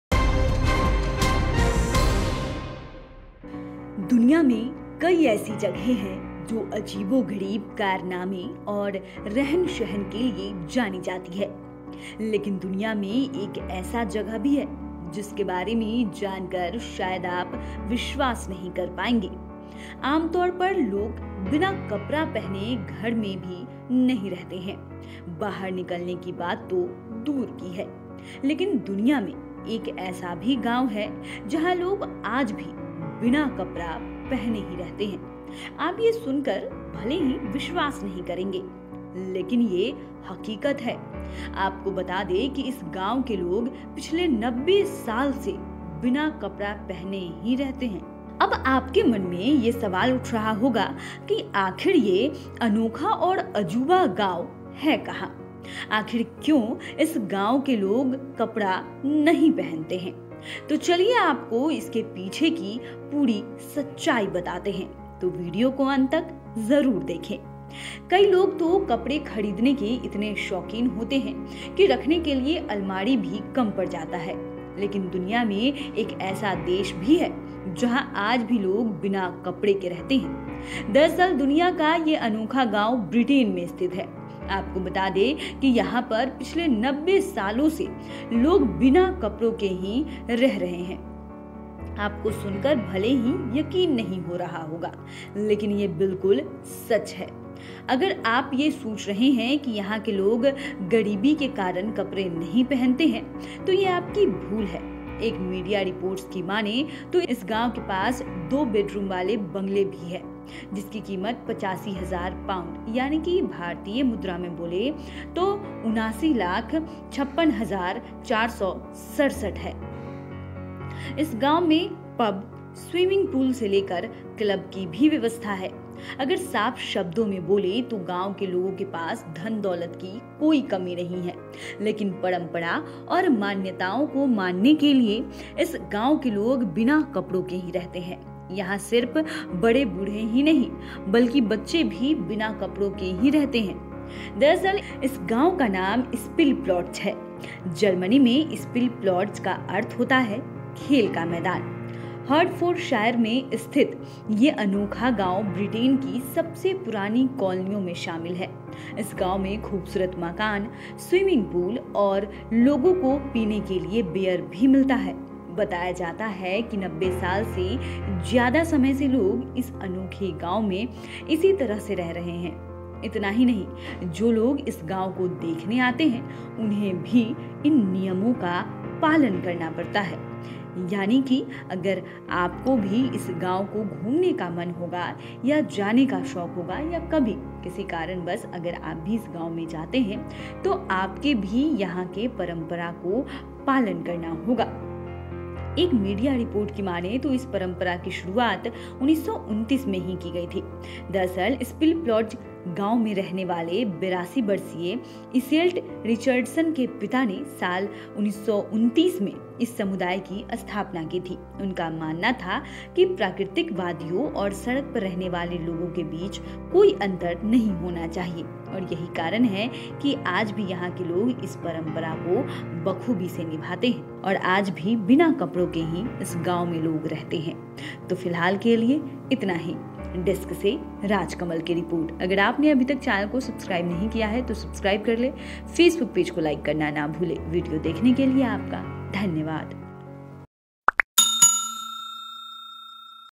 Unique Village: इस गांव में बिना कपड़ा पहने ही रहते हैं लोग, जानिए क्या है इसके पीछे की वजह
ये है दुनिया का सबसे अनोखा गांव, यहां भूलकर भी नहीं पहना जाता है कपड़ा, जानिए असली वजह, देखिए वीडियो...

Unique Village: इस गांव में बिना कपड़ा पहने ही रहते हैं लोग, जानिए क्या है इसके पीछे की वजह | Bihar News | Bihar InShots